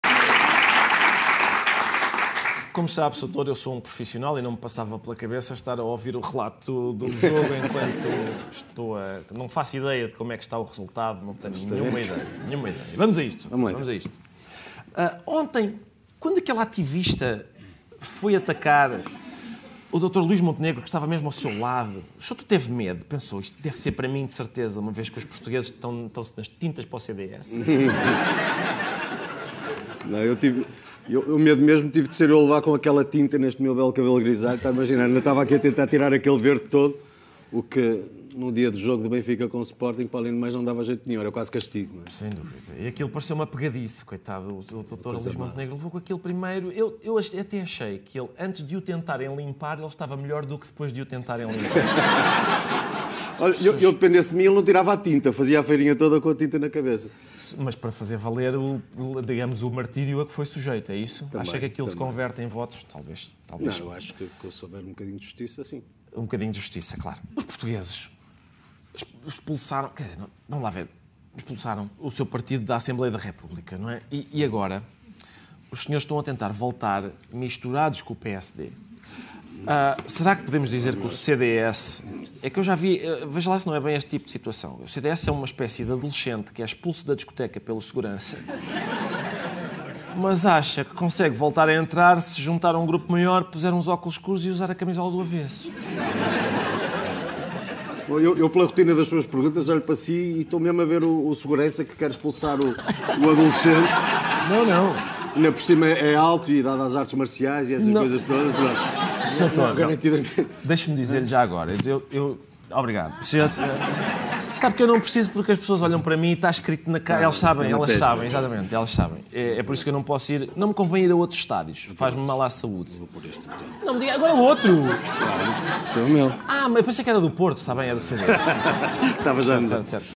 "O CDS é uma espécie de adolescente que é expulso da discoteca" - Veja a resposta de Nuno Melo
Será que Nuno Melo acha que o CDS consegue voltar à assembleia se puser uns óculos escuros e a camisola do avesso?